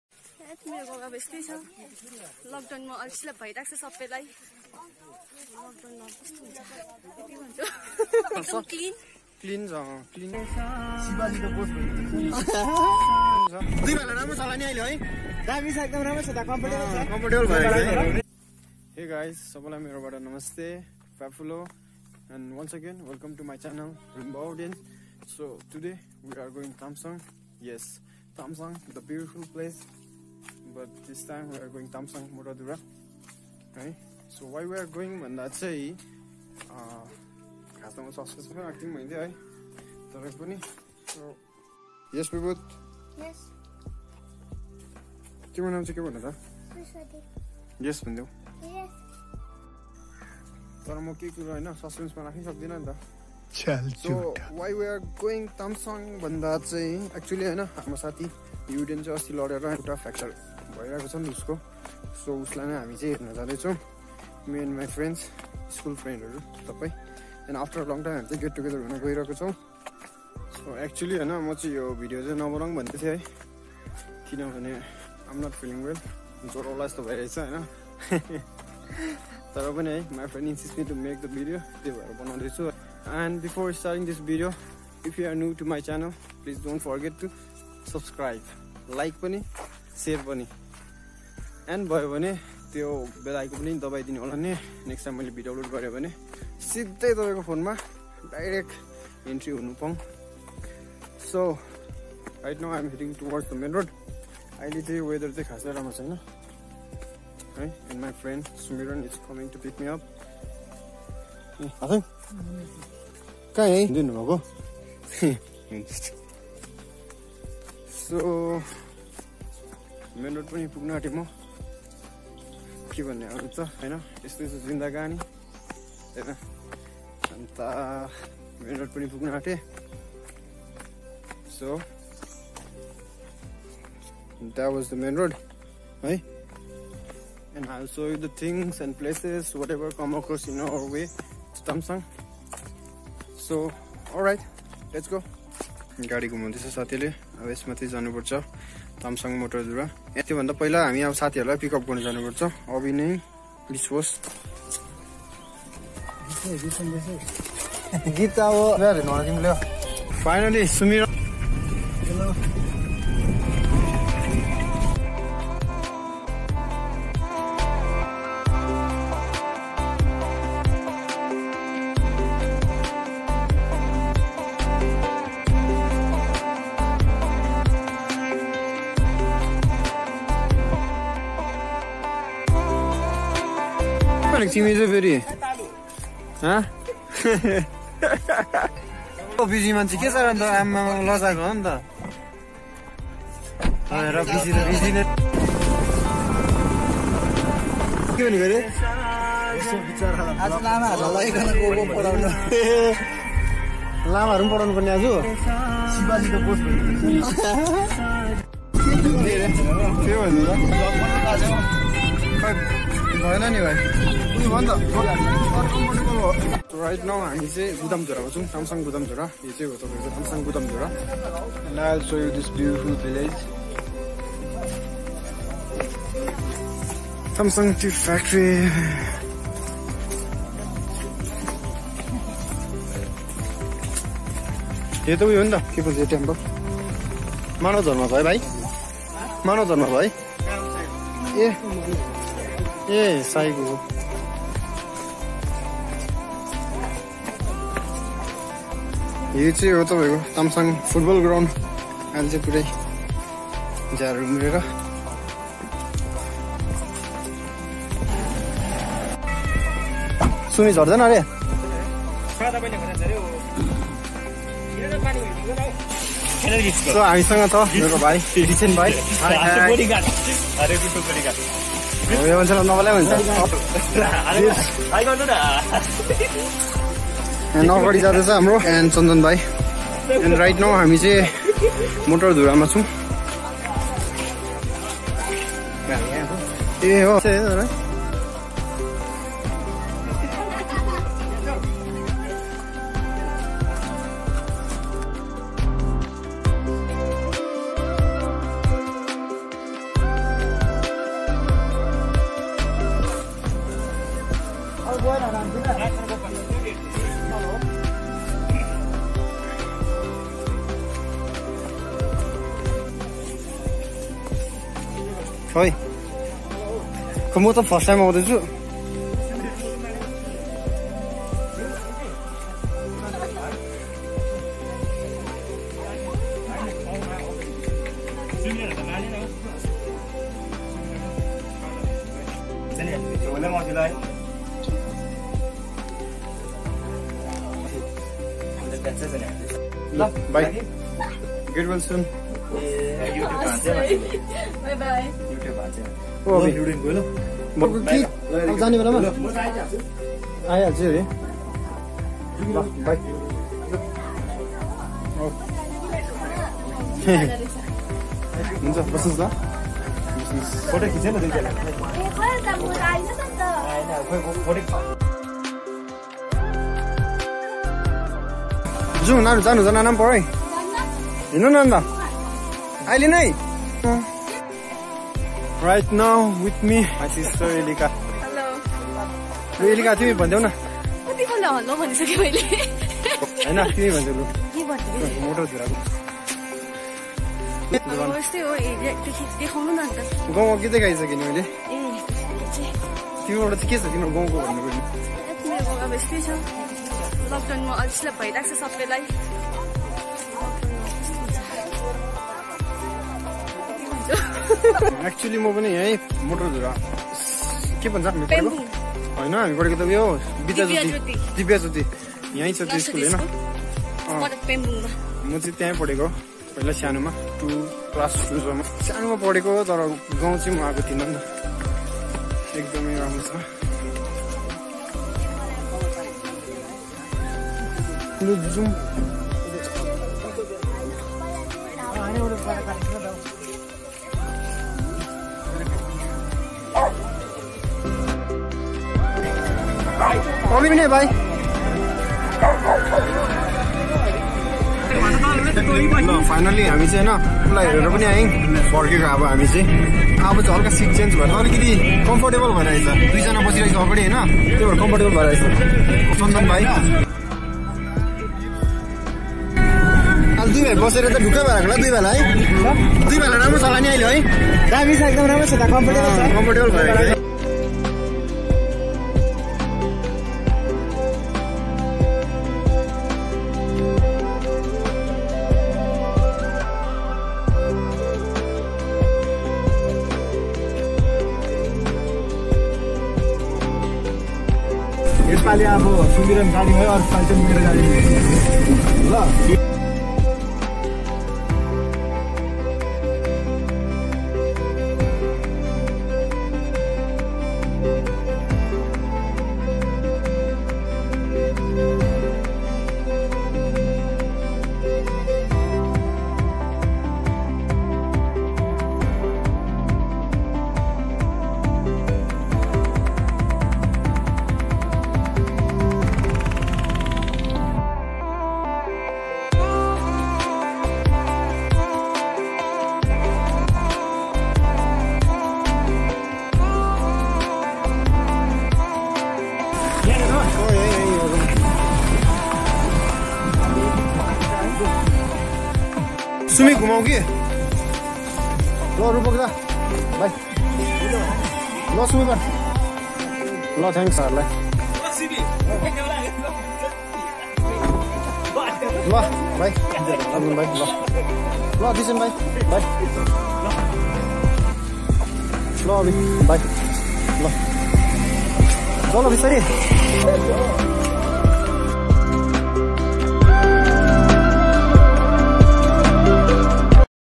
Get well soon uden/Villagevlog/Tumsong Motadhura 😍 /Rimbaudians/ Vlog 13
we all classmate went to see uden, and after a long time we all having a good time
Get well, get better,
be strong that is what we prayed to God 🙏
may God bless you uden